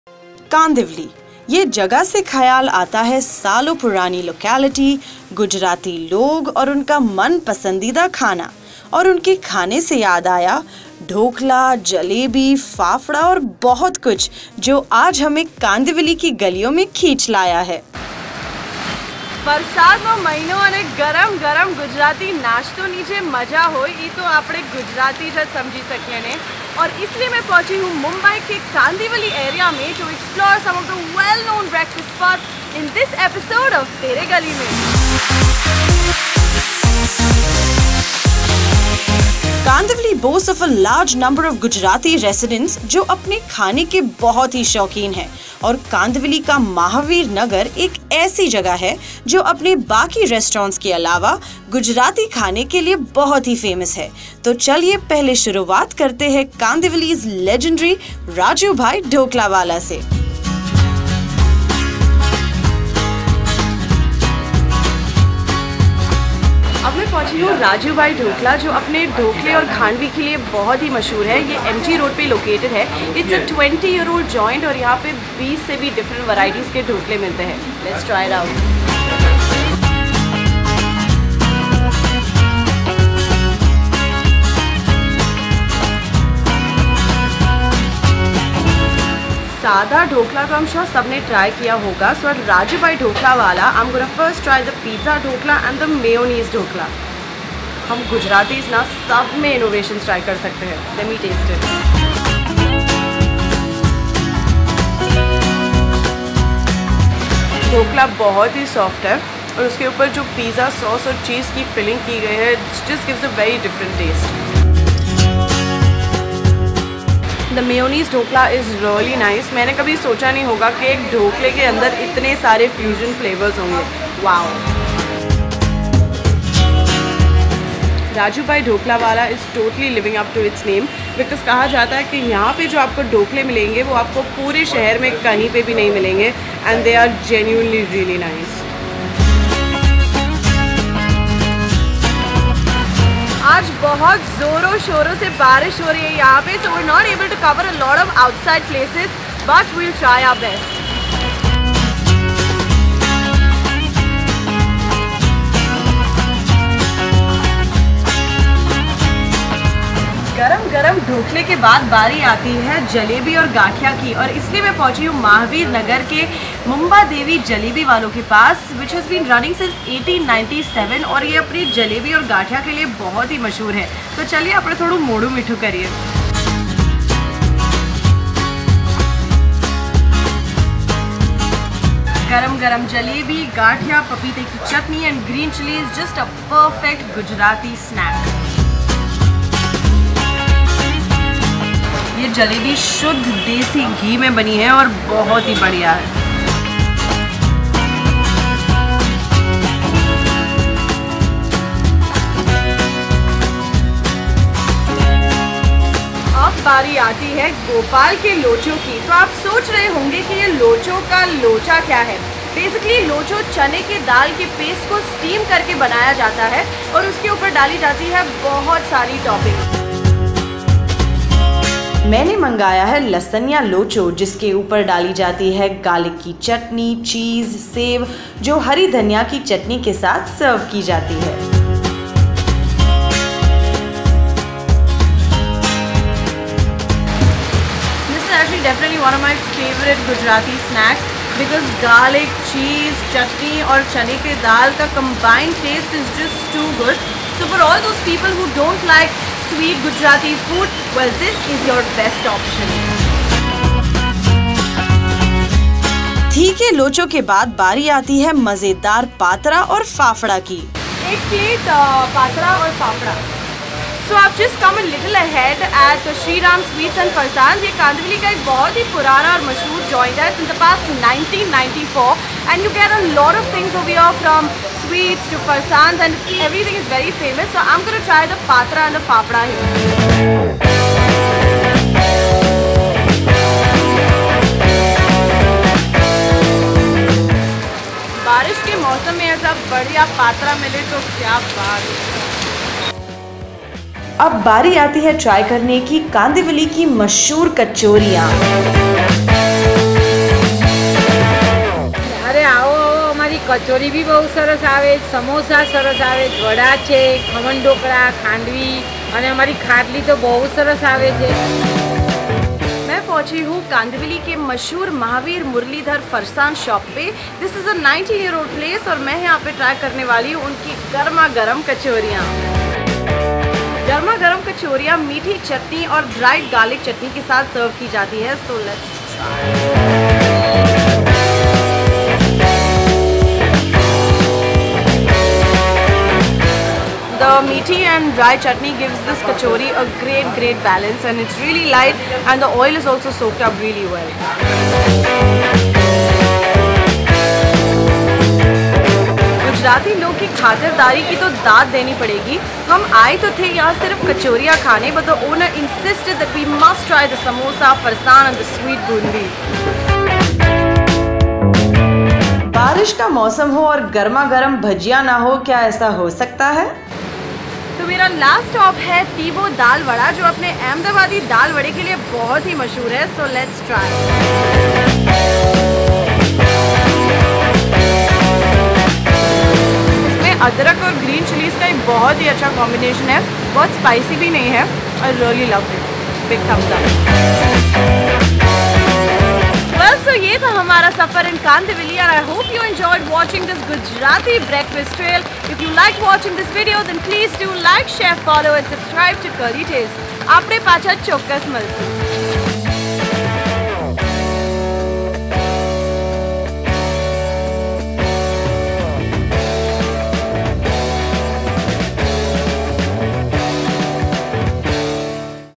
Tere Gully Mein Ep 16 - Ultimate Gujarati Breakfast Trail - Kandivali, Mumbai | Curly Tales
In the 16th episode of Tere Gully Mein, we are heading to Kandivali, which is resident to a large number of Gujarati people and we are taking you through some of the well known Gujarati breakfast spots in and around Mahavir Nagar. From Dhokla to Jalebi and Farsan, Kandivali is the ultimate place for all your Gujarati delicacies

1. Rajubhai Dhoklawala
Address:  Shop No. 7,8,9, Om Sai Ratna Rajul, Opposite Kamla Nagar, Mahatma Gandhi Road, Mumbai, Maharashtra 400067

2. Mumbadevi Jalebiwala
Address: 49 Raj Arcade,opp D Mart, Boraspada Rd, Mahavir Nagar, Kandivali, Mumbai, Maharashtra 400067

3. Gopal Locho
Address: Shop Number 42 , Raj Aracade , Opposite D -Mart, Boraspada Rd, Mahavir Nagar, Kandivali, Mumbai, Maharashtra 400067

4.Shree Ram Sweets and Farsan
Address: Raj Arcade, Shop No. 43 & 44, road, Poisar, Mumbai, Maharashtra 400067

5. Murlidhar Farsan
Address: Shop No: Ruby Classic, Opp.Jain Mandir,, 1, Mahavir Nagar, Dahanukar Wadi, Kandivali, Mumbai, Maharashtra 400067

6. Tibo Dalwada
Address: 61, Damu Anna Date Marg, Mahavir Nagar, Panchsheel Garden, Mahavir Nagar, Kandivali, Mumbai, Maharashtra 400067